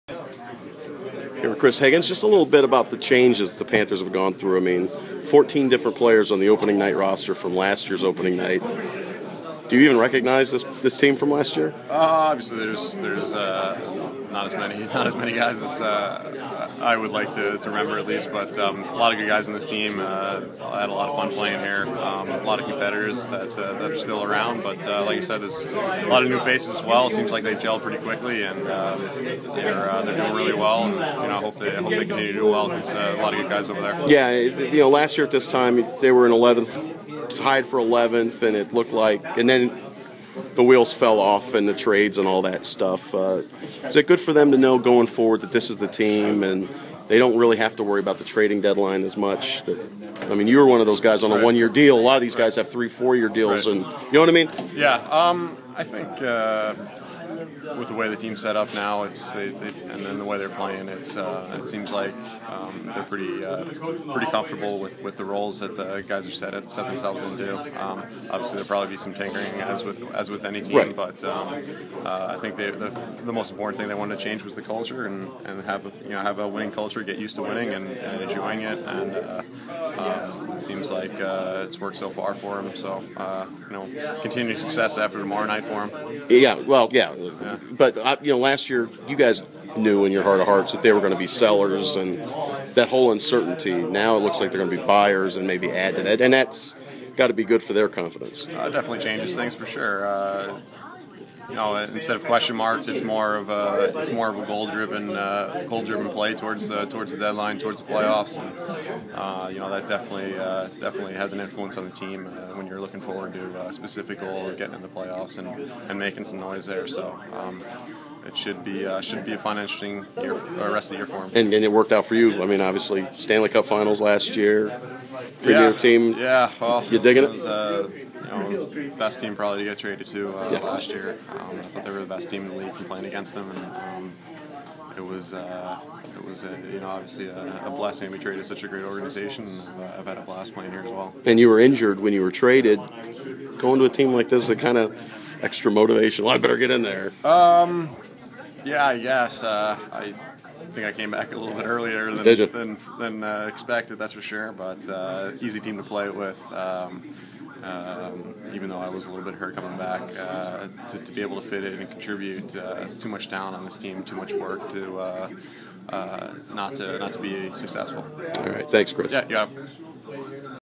Chris Higgins: Back in Sunrise
Former Panthers forward Chris Higgins talks about his old team.